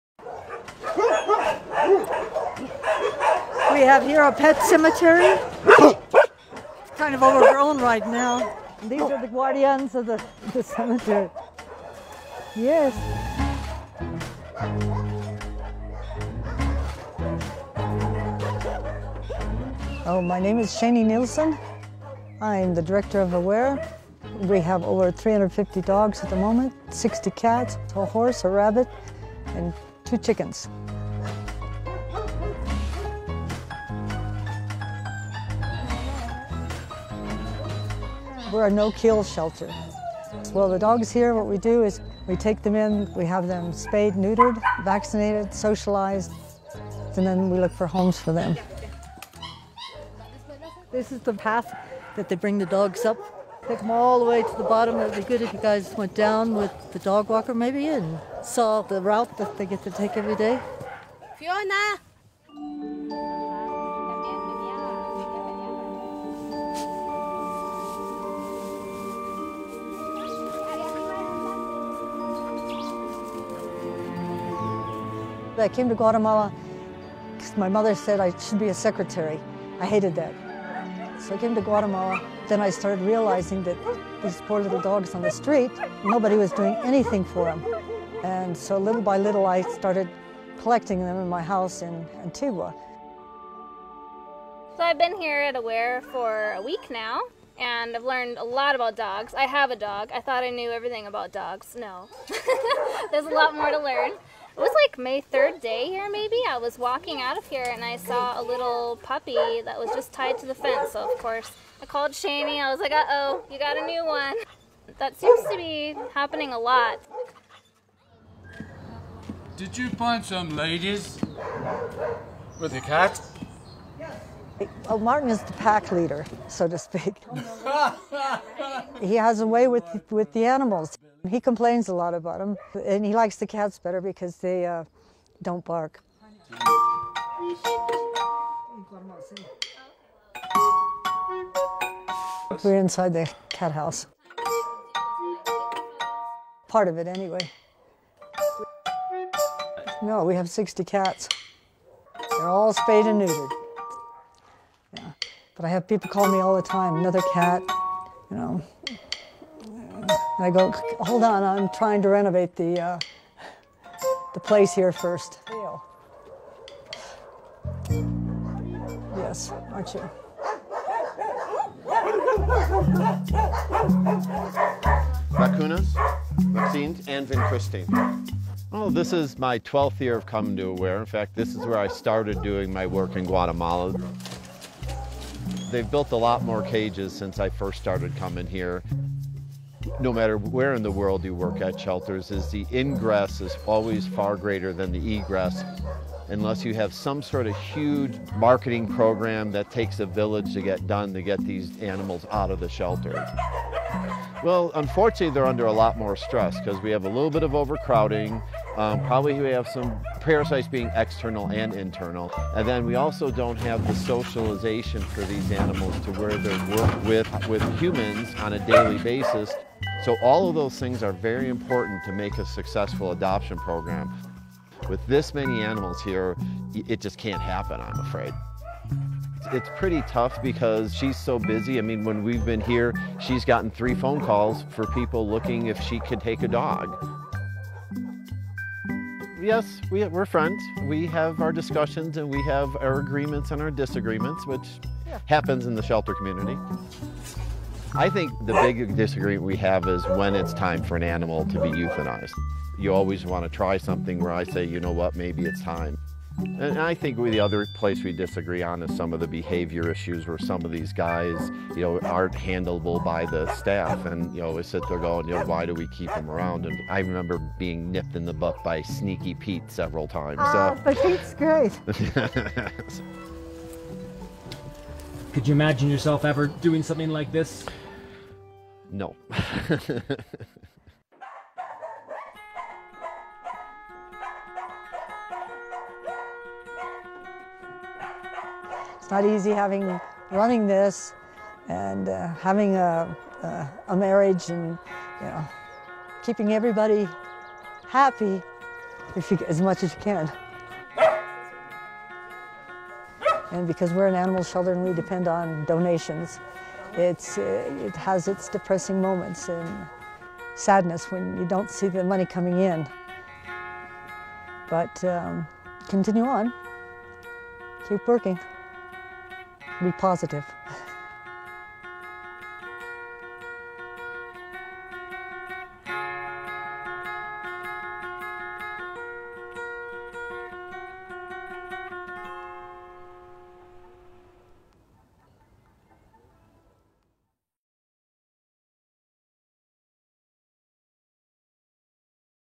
Animal Aware - Documental 2017
Animal AWARE (Animal Welfare Association - Rescue/Education) is a non-profit, non-governmental charitable organization for the rescue and welfare of abandoned or abused animals. It was founded in 1998 in Guatemala by Xenii Nielsen (USA), Gina Illescas (Guatemala), and Pamela Hirst- Prins (England). Our mission is to reduce the suffering and improve the lives of domestic animals in Guatemala.